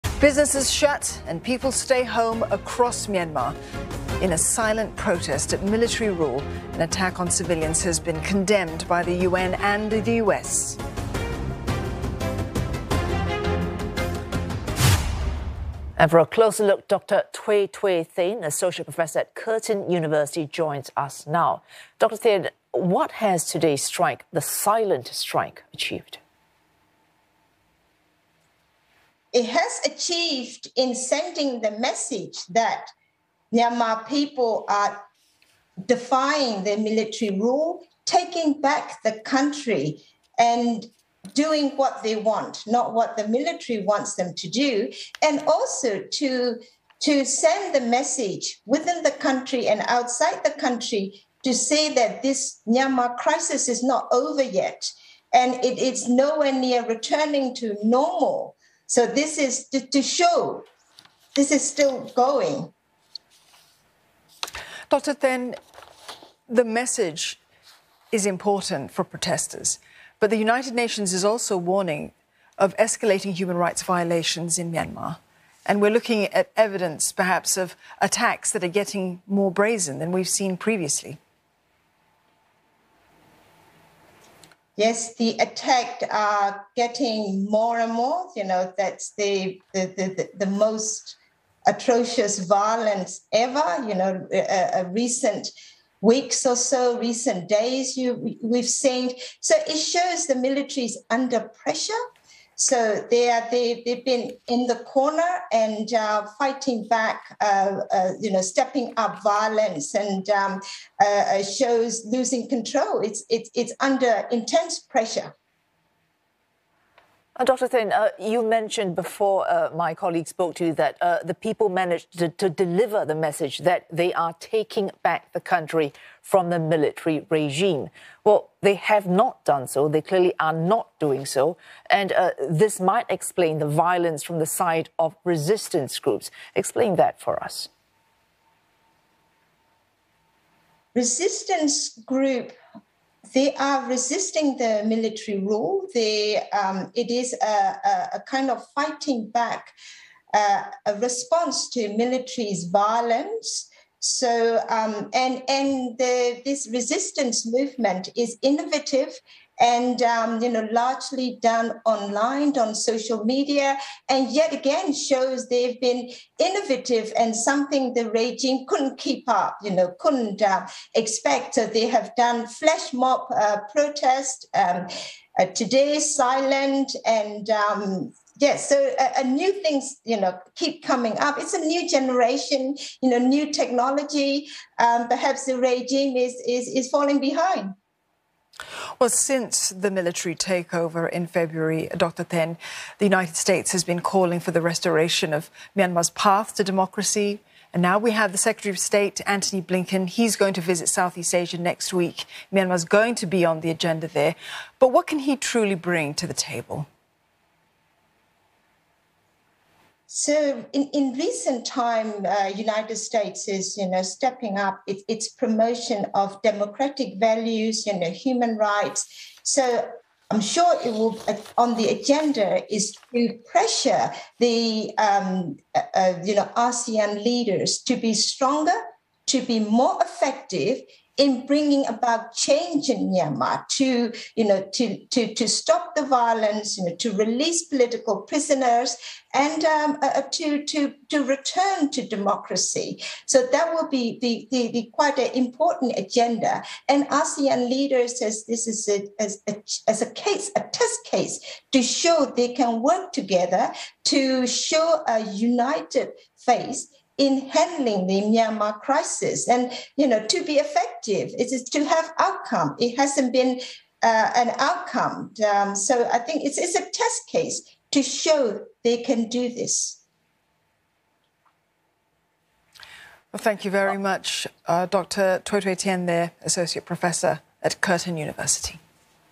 Assoc Prof Htwe Htwe Thein on the 'silent strike' against Myanmar's military govt
Across Myanmar, businesses have shut and people are staying home as part of a "silent protest" against military rule. Soldiers have been accused of killing 11 civilians and burning their bodies this week. The UN and US have condemned the attacks on civilians. Dr Htew Htew Thein from Curtin University shared her thoughts on the situation in Myanmar and US Secretary of State Antony Blinken's visit to Southeast Asia next week.